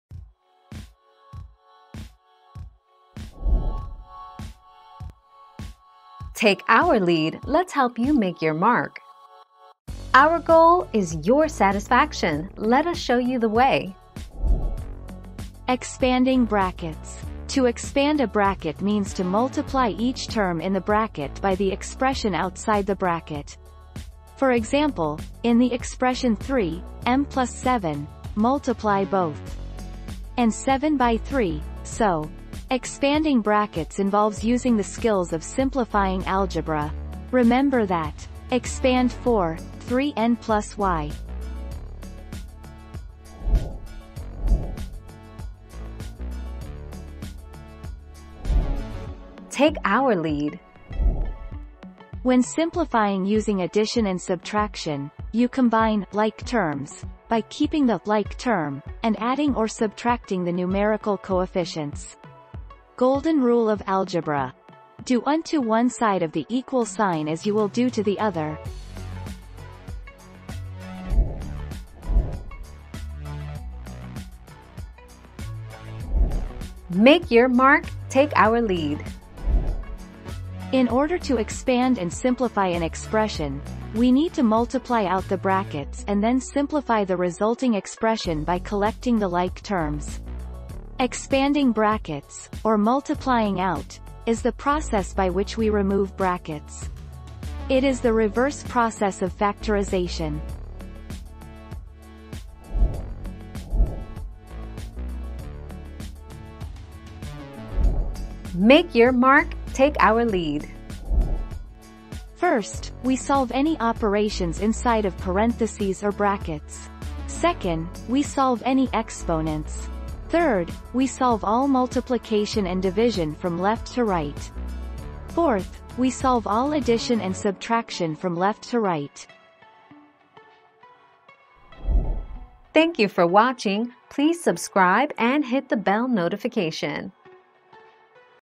How do you simplify expressions with brackets?
00:00 - How do you simplify expressions with brackets?
00:45 - How do you simplify like terms?
01:12 - How do you expand and simplify an equation?
01:44 - Do You Solve brackets or parentheses first?

Laura S. Harris (2021, August 27.) How do you simplify expressions with brackets?
    AskAbout.video/articles/How-do-you-simplify-expressions-with-brackets-260926